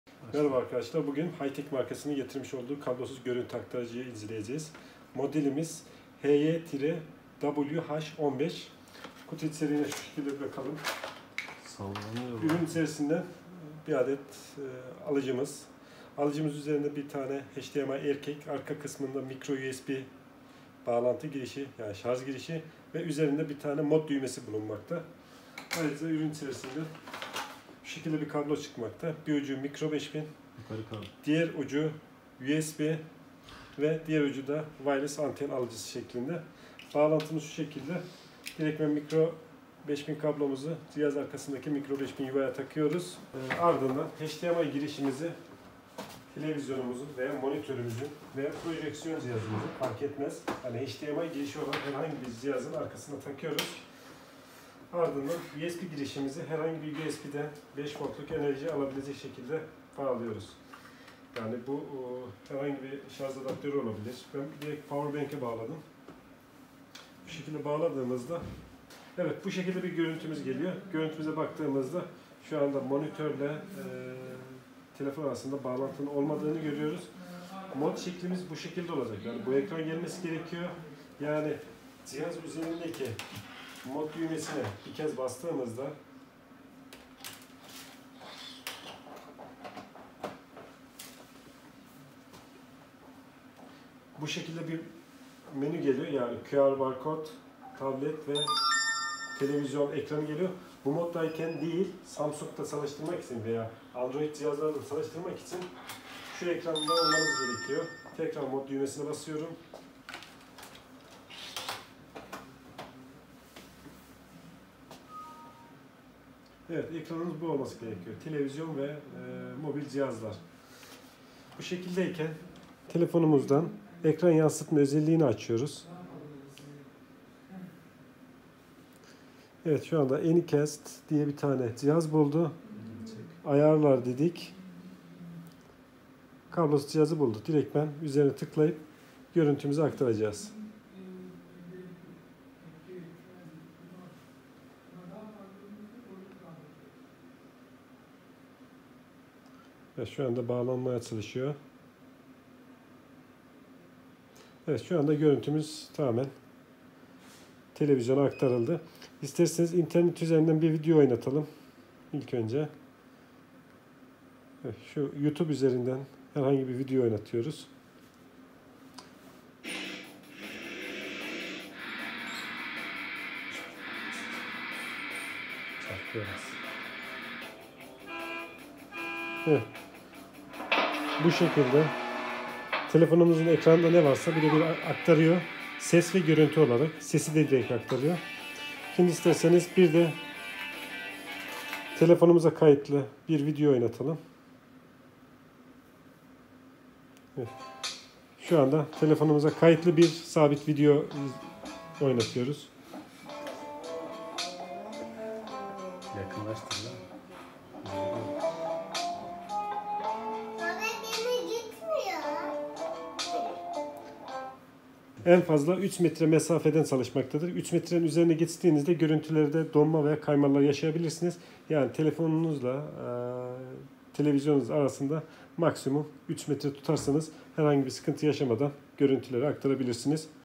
HYTECH HY-WH15 Kablosuz HDMI Görüntü Aktarıcı (Android)
HYTECH HY-WH15 kablosuz görüntü aktarıcı Android cihazının kurulum ve incelemesini yaptım. inşallah beğenmişsinizdir.